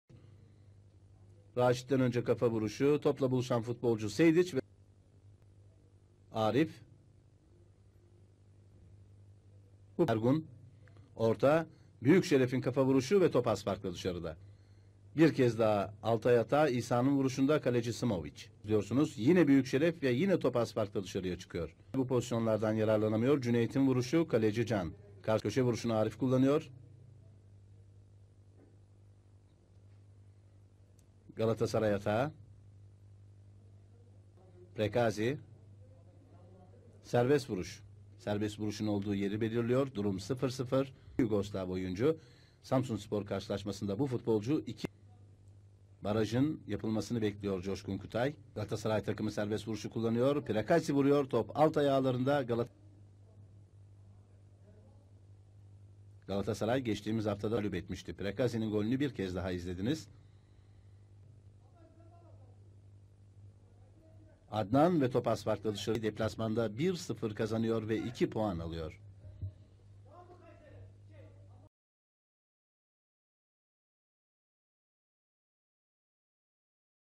1985 1986 Altay Galatasaray 2.Hafta Maçı
altay: can barhan, zafer bilgetay, önder bilgin, miodrag jesic, ergun ortakçı, isa ertürk, şeref onarlıoğlu, müjdat atıcı, mirsad sejdic (dk. 46 adnan dürüst), erdi demir, emin uslu
teknik direktör: kemal omeragic (yugoslavya)
galatasaray: zoran simovic, halil ibrahim akçay (dk. 85 öner kılıç), raşit çetiner, erhan önal, sefer karaer (dk. 53 adnan esen), cüneyt tanman, ahmet ceyhan, yusuf altıntaş, arif kocabıyık, dzevad prekazi, bülent alkılıç
teknik direktör: jupp derwall (batı almanya)
gol:
(0-1) dk. 62 dzevad prekazi
sarı kart:
dk. 19 emin uslu, dk. 61 miodrag jesic, dk. 83 isa ertürk (altay)
dk. 38 dzevad prekazi (galatasaray)